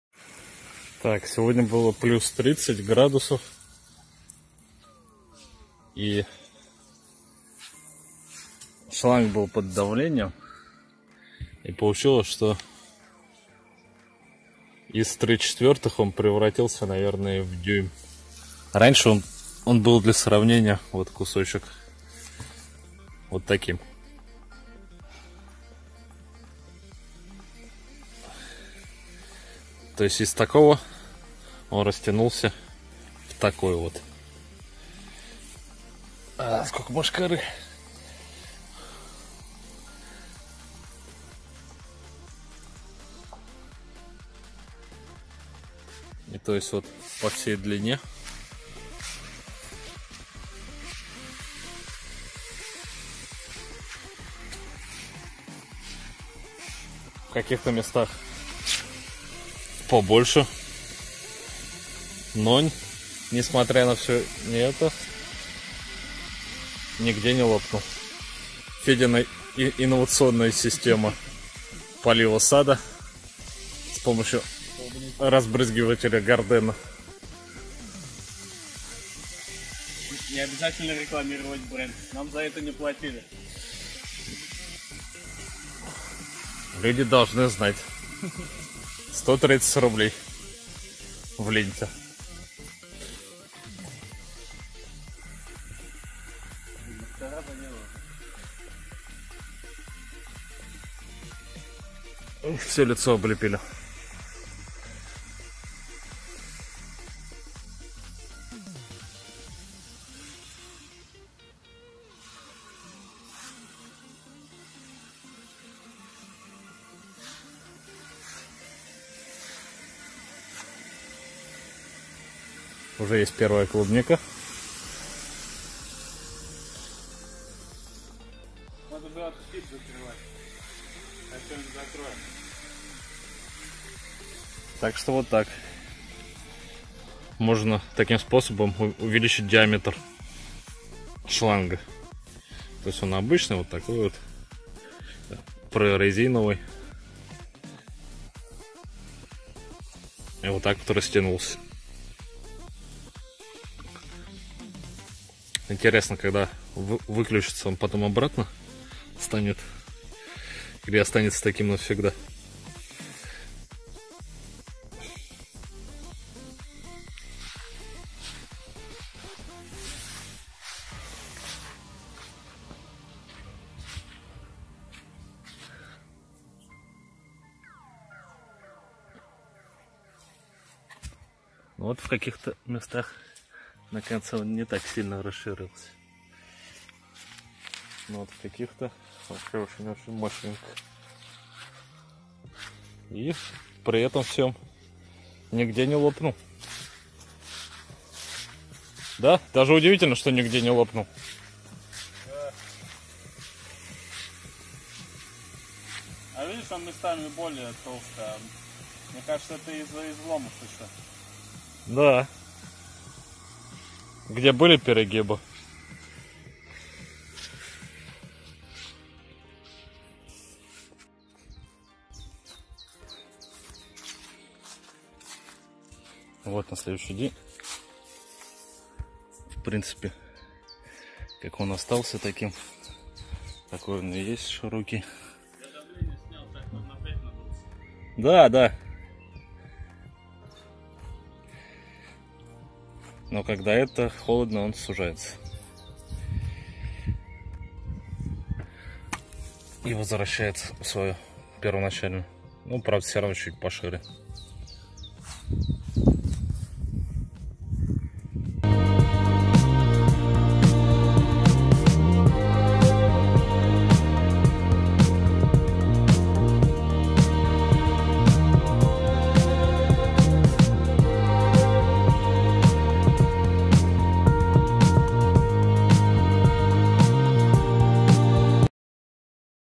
Что будет если дешёвый садовый шланг оставить под давлением в жару ?!
Денёк был весьма жаркий. Садовый шланг для полива огорода остался под давлением от насосной станции. Давление 3 атмосферы хватило, чтоб шланг из диаметра в 3/4 превратился в дюйм. При этом, как не странно, не одного прокола не произошло.  Потом за ночь он немного вернулся к первоначальному состоянию.